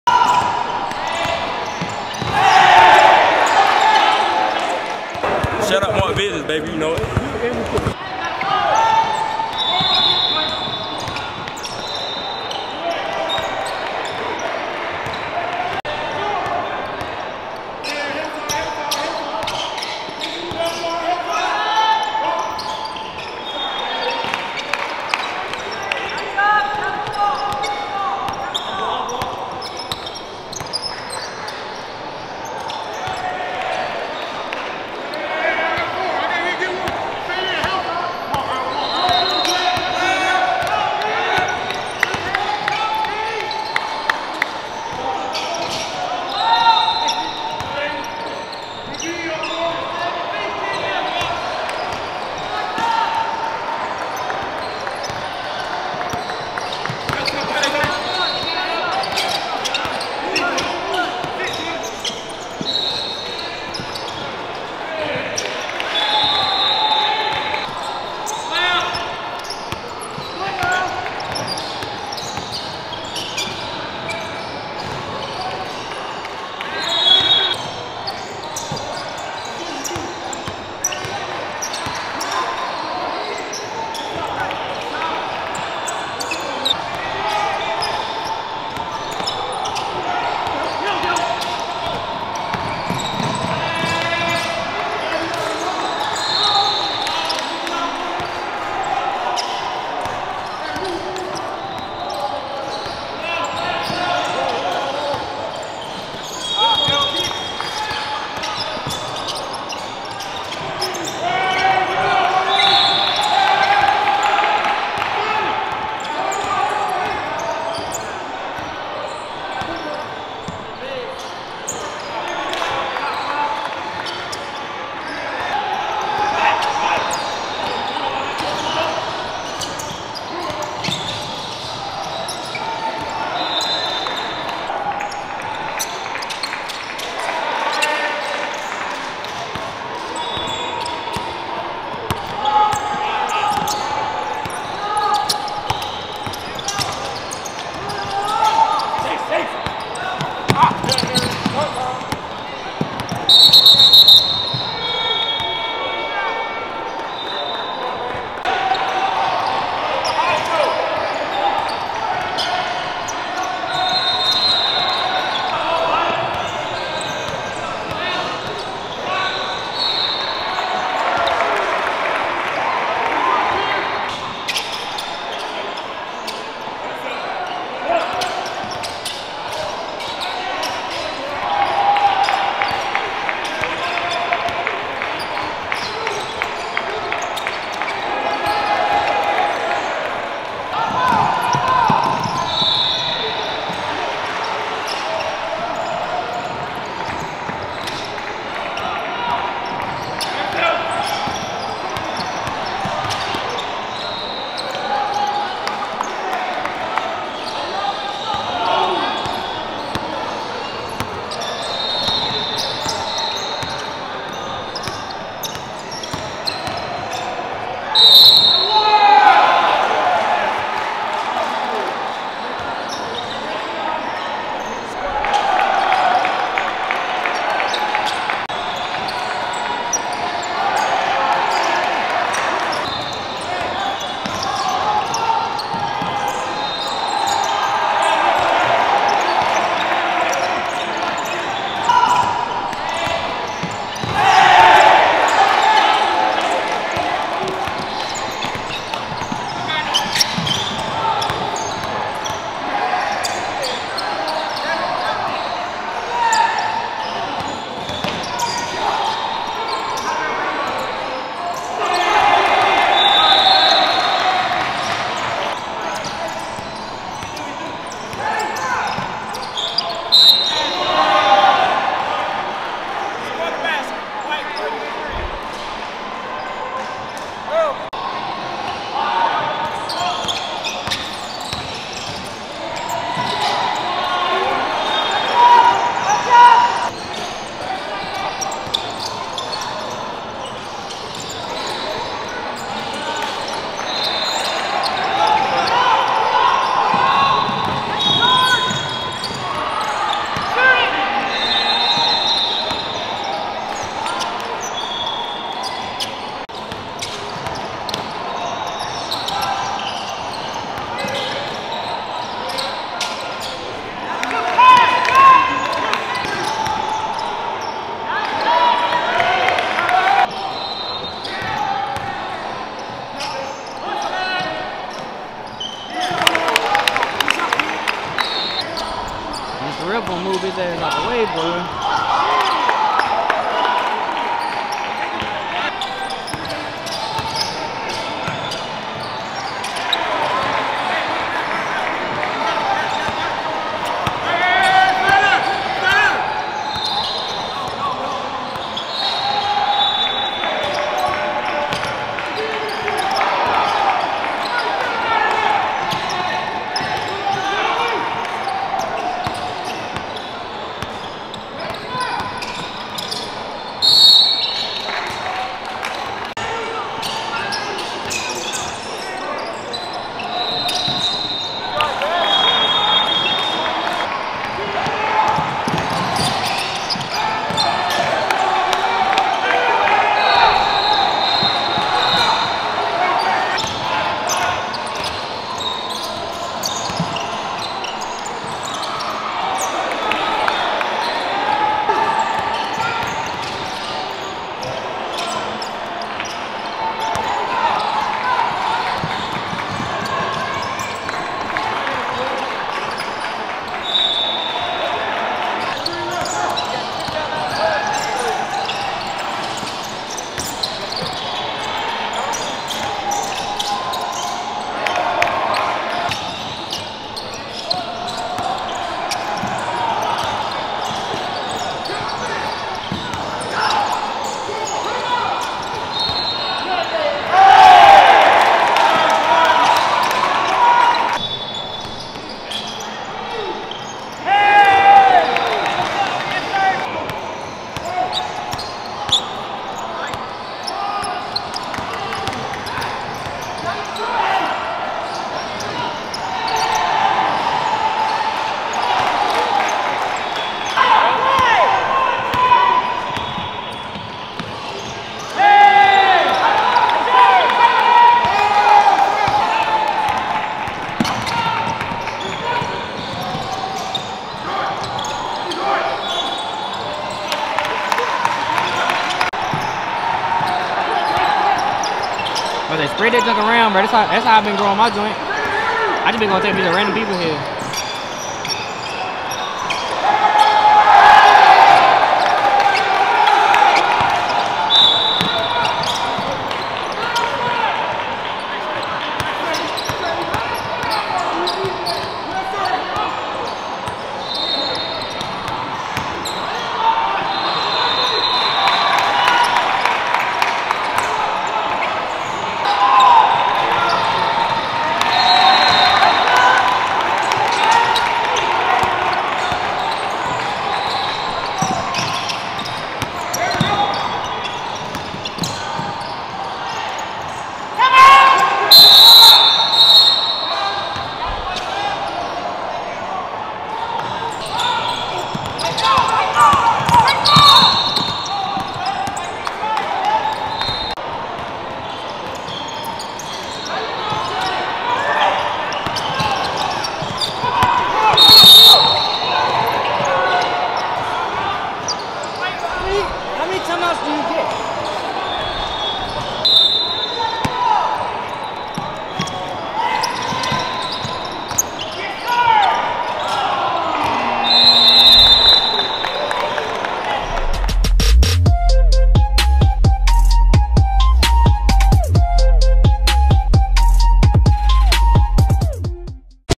HSH 🎥🏀 | Dorman vs Chapin | Upward Stars Fall League | Reigning State Champions go to the wire!
Dorman High went down to the wire with Chapin High at the Upward Stars Fall League! How do you think the Cavaliers are looking? They played the game without junior big man Noah Clowney and still came out with the win.  Dessie Canty Jr will be a huge addition to the team!